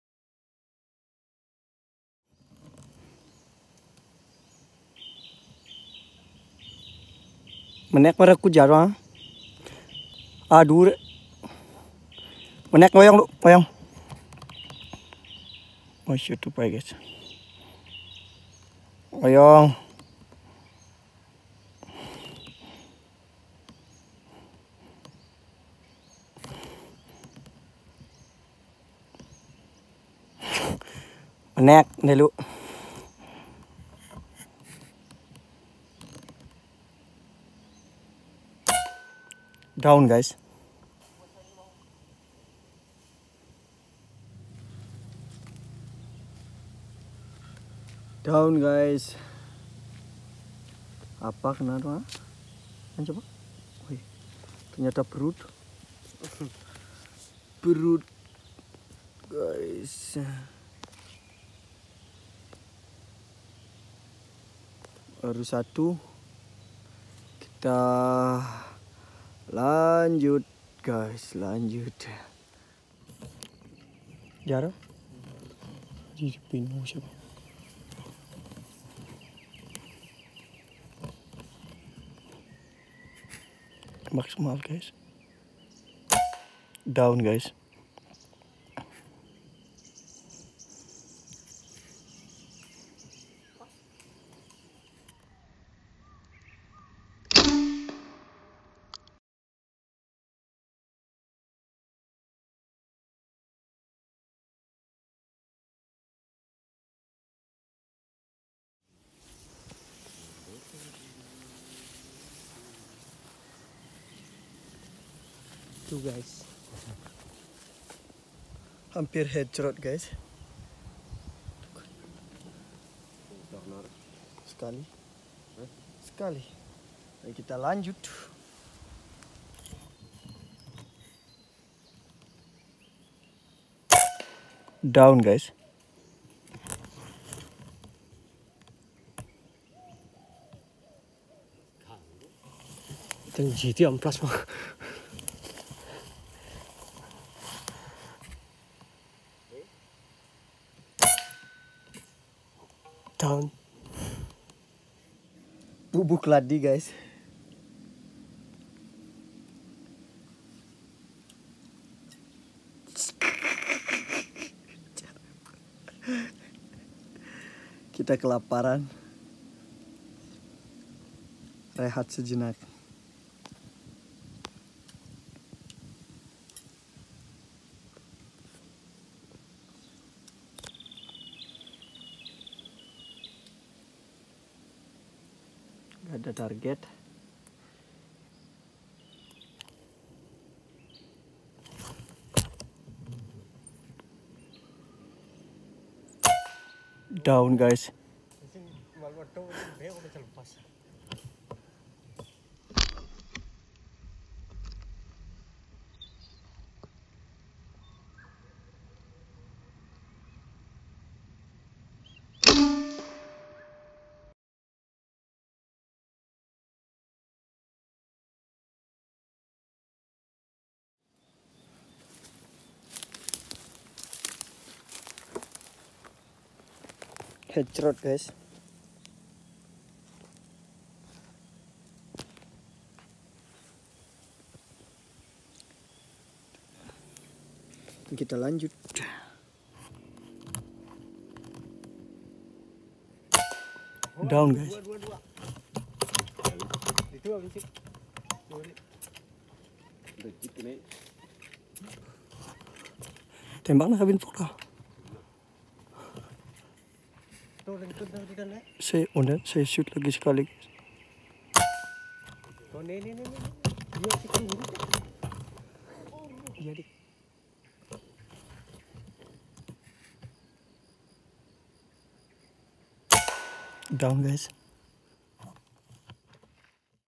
Ke hutan lagi setelah lama off || berburu tupai || huntingvlog
Kami bisa ke hutan lagi setelah beberapa hari off karena ada acara pitra yadnya (ngaben)